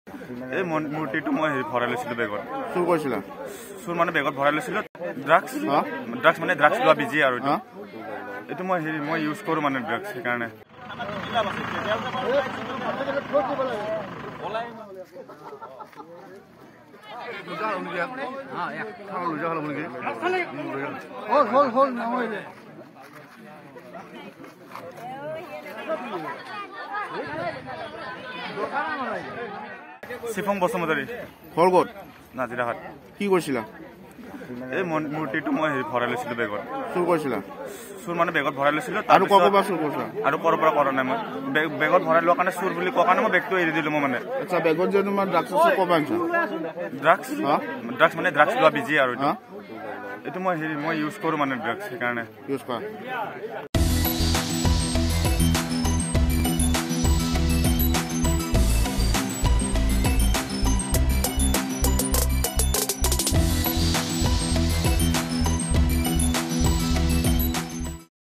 মন্দিৰত মূৰ্তি চুৰি কৰিবলৈ লওঁতে যুৱকক ৰাইজে দিলে লপা-থপা
▶️ড্ৰাগছ আসক্ত চোৰৰ বিলৈ। মন্দিৰত মূৰ্তি চুৰি কৰিবলৈ লওঁতে ৰাইজে দিলে লপা-থপা। উদং গাৰে বহুৱাই ভগৱানৰ উপাসনাস্থলীত হাত দিয়াৰ অভিযোগত দিলে এশিকনি। ঘটনা সোণাপুৰৰ নাজিৰাখাটৰ।